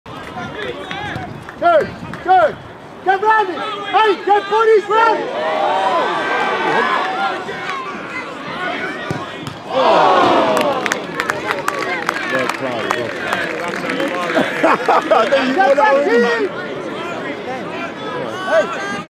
PK Humble leaving man in the mud! 👀🤷🏿‍♂️
Taken from Cheshunt FC vs Enfield Town

Head over to Sk1pr website to purchase some amazing kits and teamwear for your grassroots football club